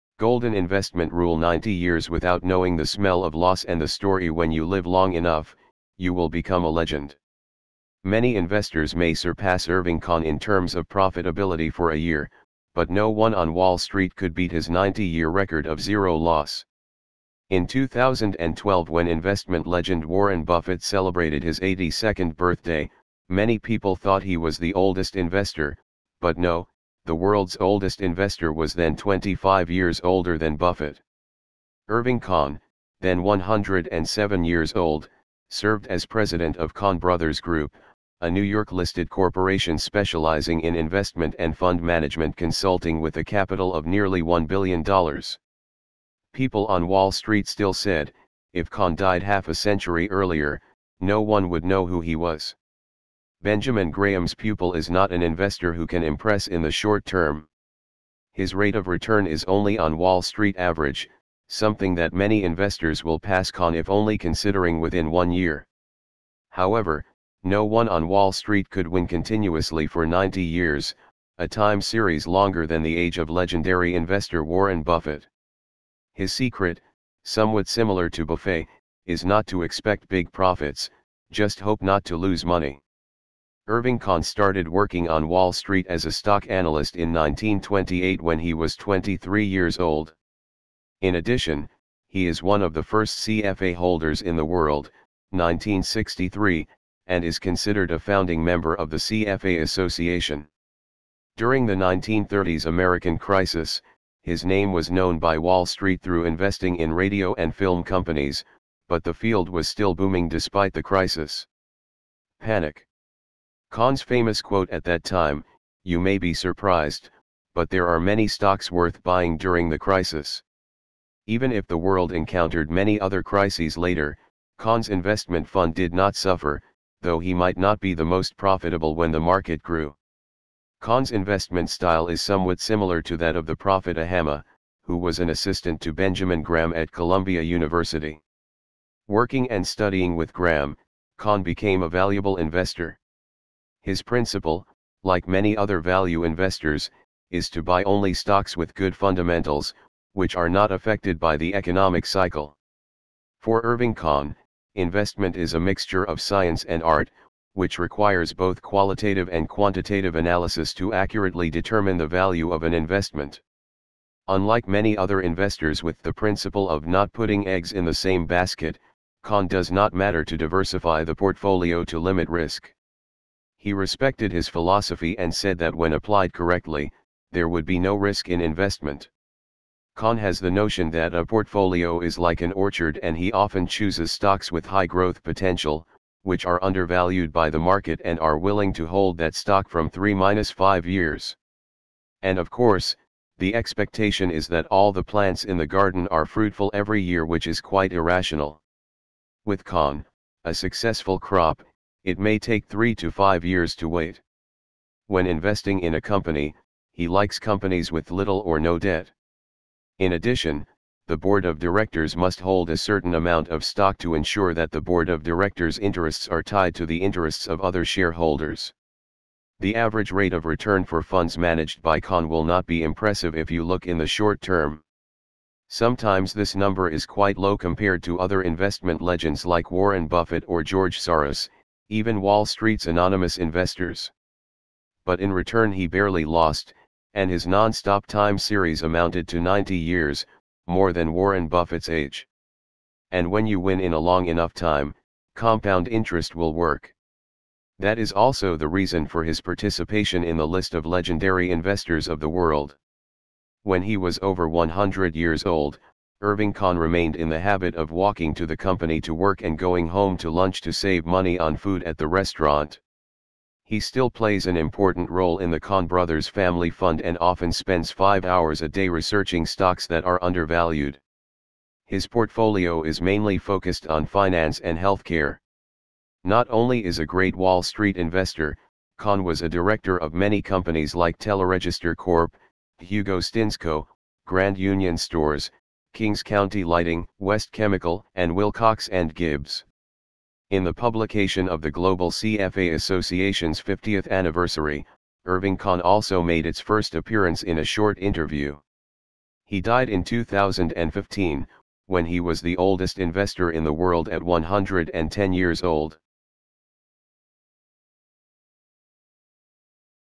Irving Kahn-90 years do not know the smell of losses and the story
[Golden investment rule] 90 years without knowing the smell of loss and the story "when you live long enough, you will become a legend"